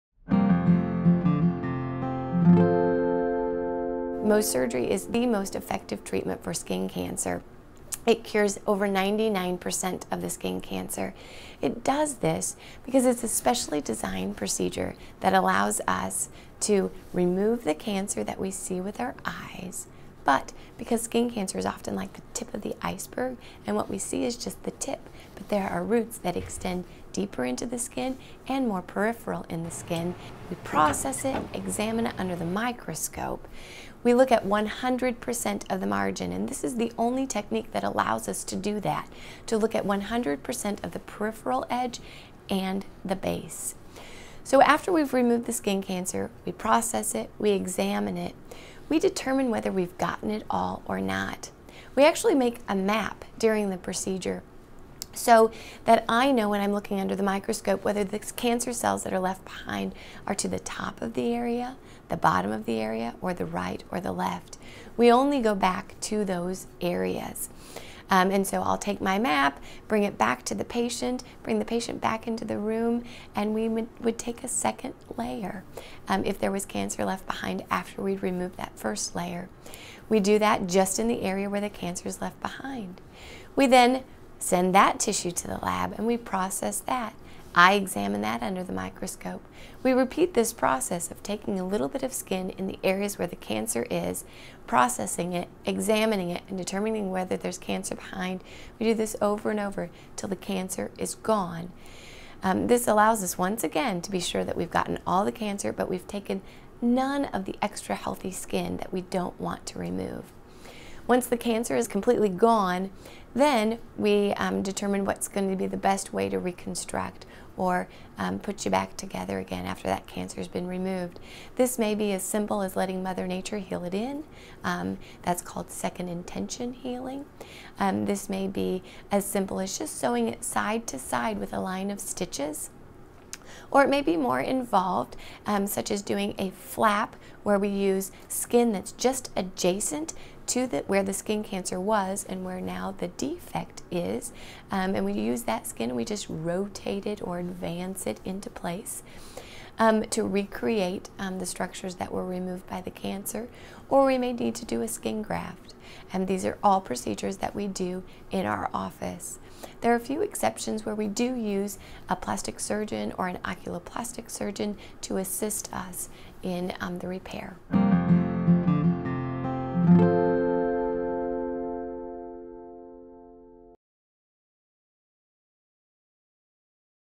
Mohs Surgery - Why it Works | McFarland Clinic
Mohs surgery is the most effective treatment for skin cancer. It allows the surgeon to remove the cancer that can be seen with the eyes and then also the roots. The technique allows the physician to get a 100% margin.  Learn more about why it works in this video.